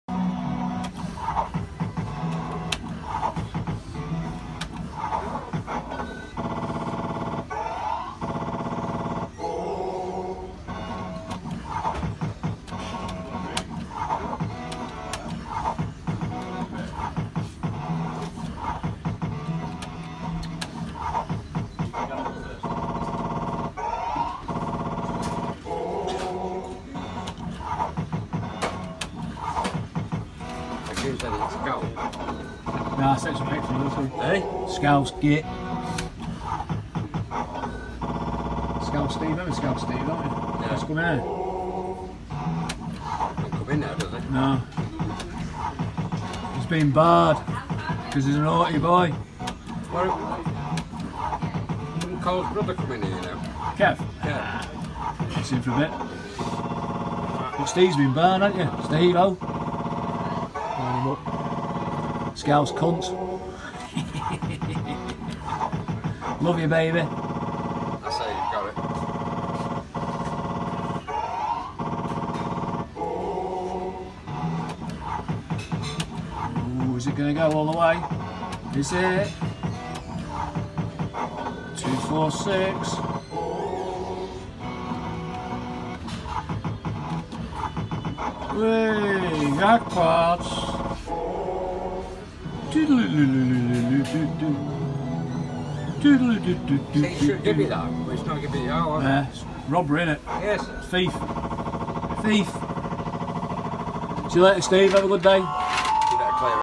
Club red hot six jackpot £250
Played by Dave penna the Stockport whopper in snooker club